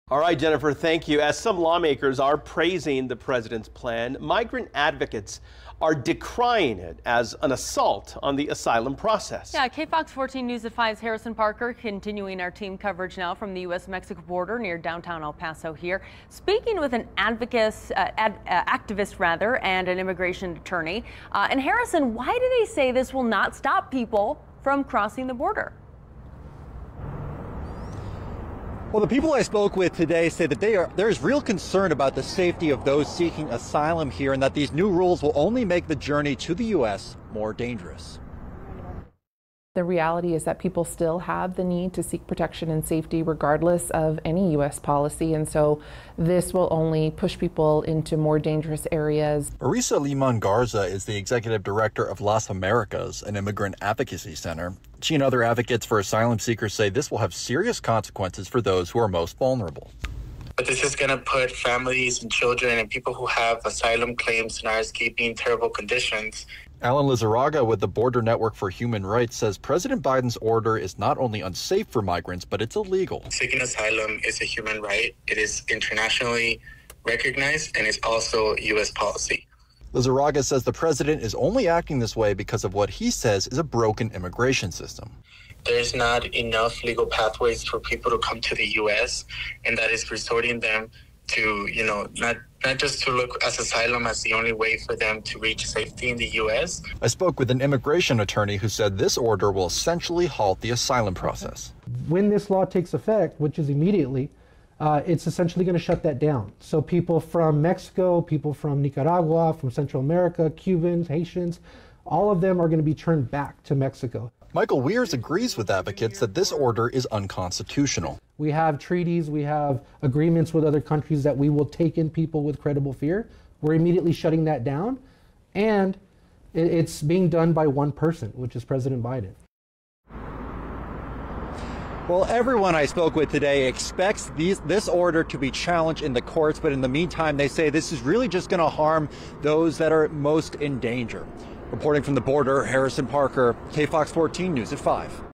Biden's new asylum cap sparks outrage; migrant advocates warn of human rights crisis
President Biden’s executive order on immigration has some Americans at the southern border are upset about what will happen to migrants seeking asylum.

Biden's order would significantly limit the number of people who can seek asylum.

Several activists and immigration attorneys explained how they think the new policy will affect those looking to cross the border.